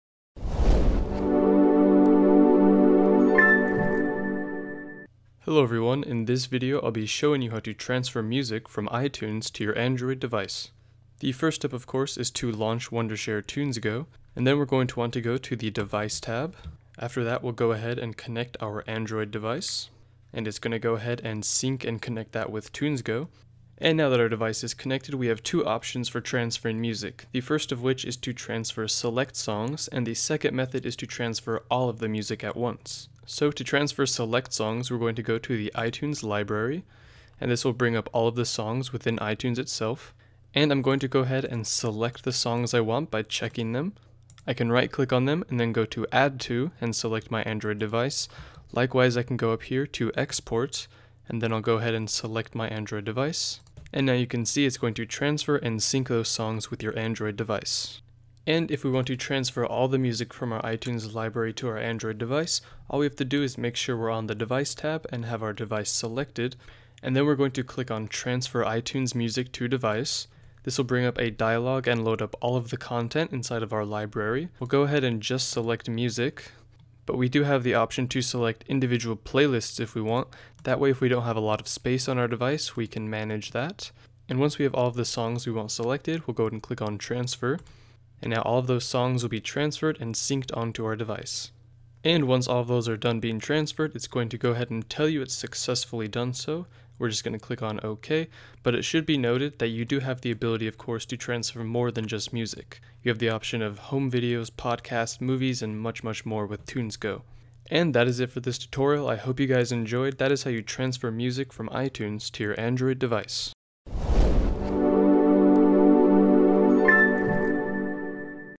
TunesGo: Transfer Music from ITunes to Android Device
Transfer music without device limitation
1. Transfer your iTunes media files (Music, Playlists, Movie, Podcasts, TV Shows, iTunes U and more) without limits.
a. iPhone/iPod/iPad ← → iTunes.
b. iPhone/iPod/iPad/Android ← → PC/Mac.
c. Android ← → iTunes.
2. TunesGo automatically converts almost any music file to a format supported by your device and iTunes. Compatibility is never an issue.